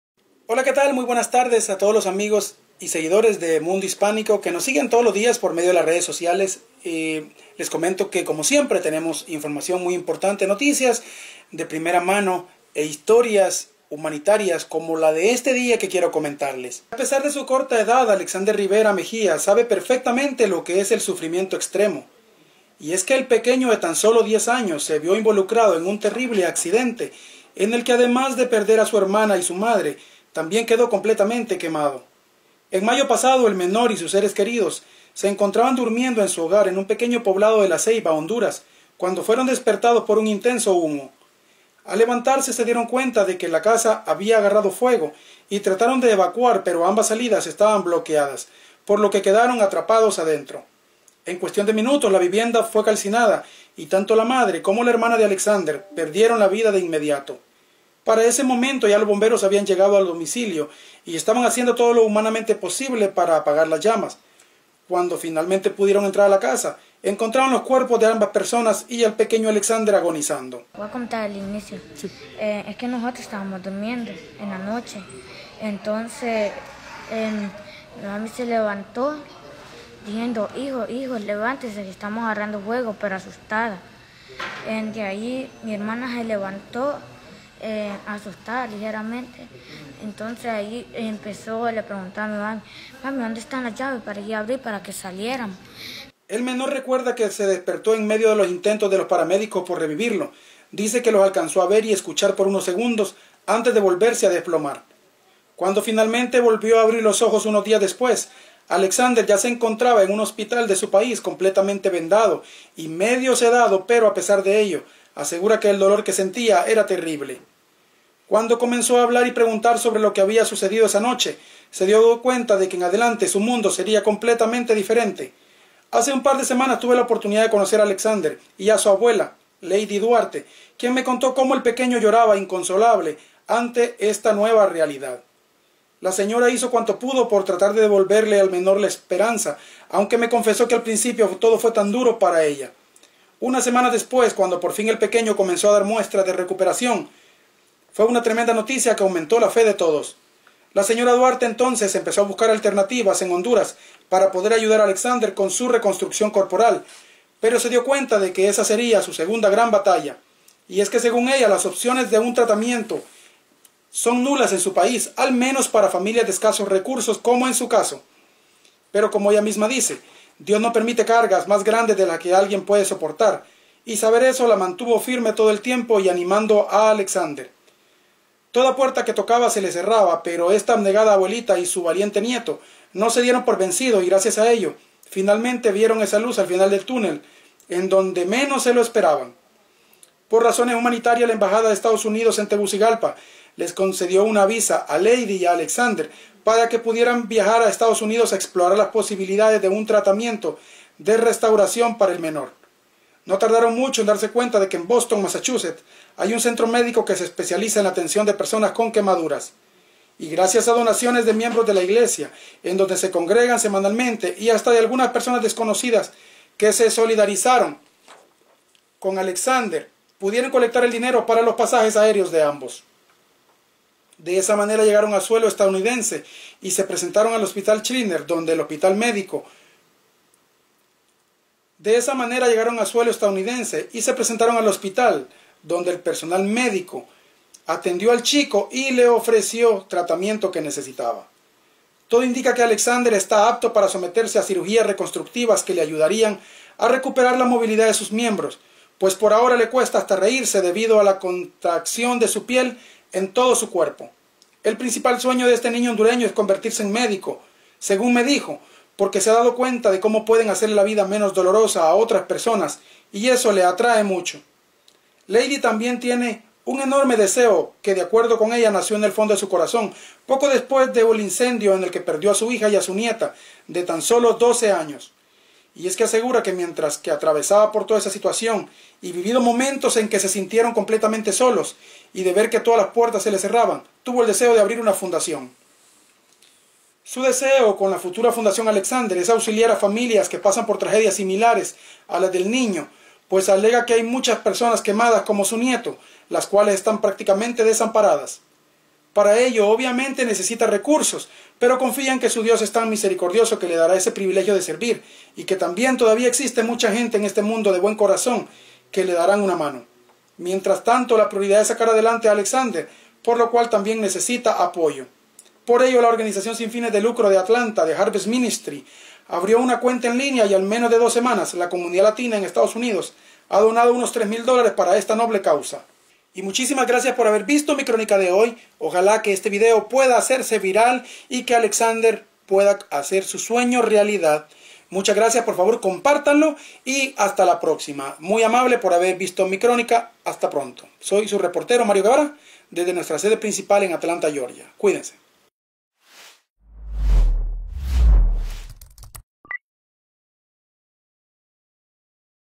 Pequeño de 10 años requiere ayuda por grave accidente que sufrió
Un pequeño de tan solo 10 años sufrió un terrible accidente en el que resulto quemado en todo su cuerpo, en este accidente en el que su vivienda fue alcanzada por fuego, Alexander Rivera Mejía, perdió a su madre y su hermano.

Su hogar se consumió en solo cuestión de minutos, el terrible accidente ocurrió en la noche mientras Alexander y su familia dormían.